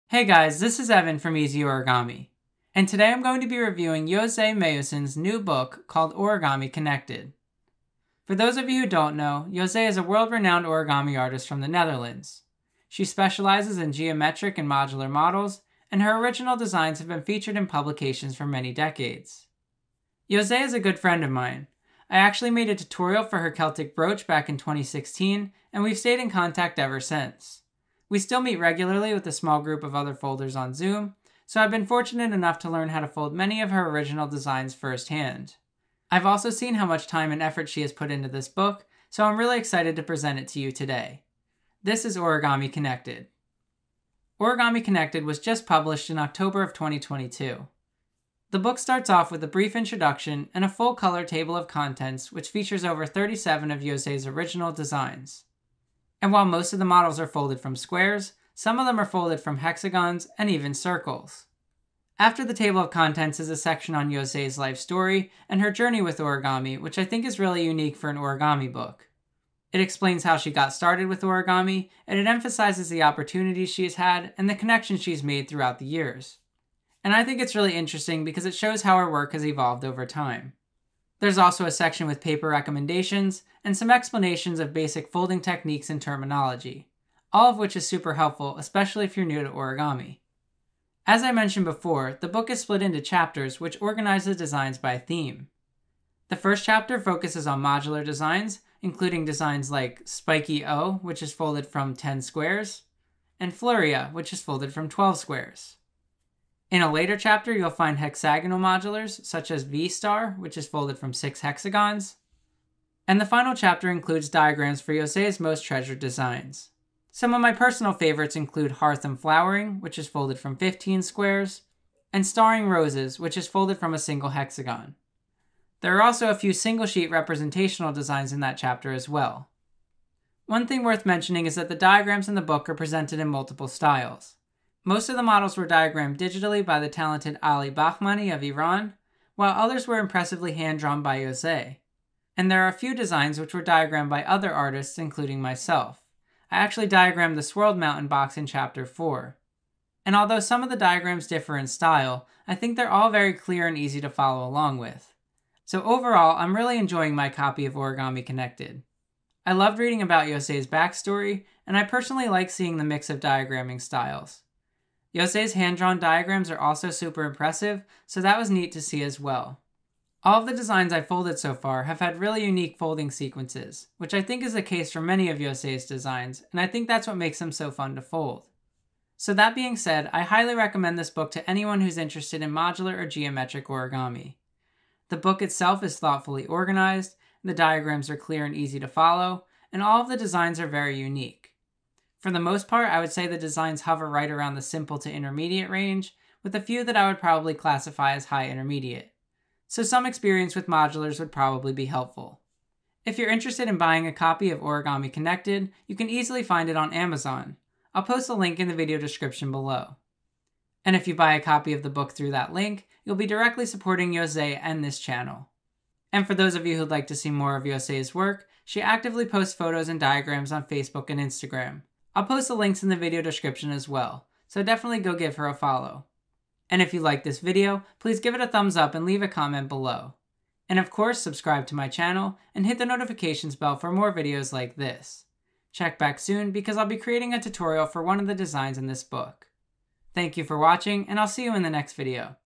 Origami Connected (José Meeusen) - Book Review
In this video, I review José Meeusen's new book, "Origami Connected". It includes diagrams for over 37 of José's original modular, geometric, and representational designs.

Congratulations to José and everyone else involved in making this book a reality! Specifically Ali Bahmani, Ilan Garibi, John Farrell, and all of my origami friends-- you did a great job. I know how important creating this book was to José, so it makes me happy to finally have a physical copy of it.

Book review by Evan Zodl. Any additional feedback is greatly appreciated! :)